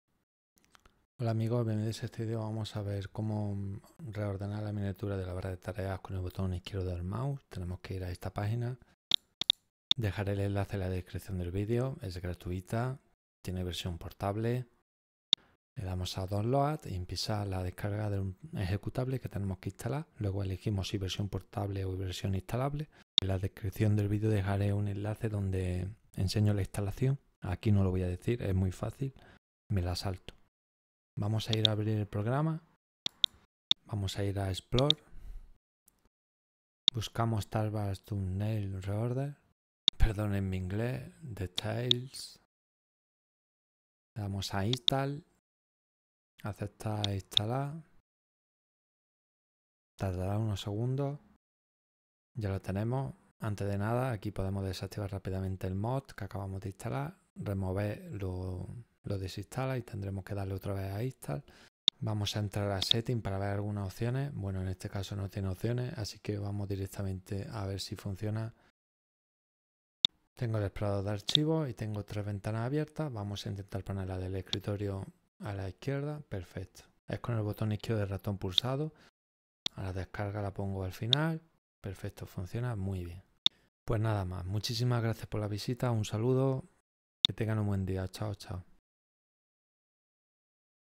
Cómo ordenar las miniaturas de la barra de tareas en Windows 11 💻 Ordenar vistas previas
Reordene las miniaturas de la barra de tareas con el botón izquierdo del mouse. Cómo ordenar vistas previas en Windows
💻 Valido para Windows 11

El programa es gratis y no hace falta que lo instales, tiene versión portable.

Taskbar Thumbnail Reorder
Reorder taskbar thumbnails with the left mouse button.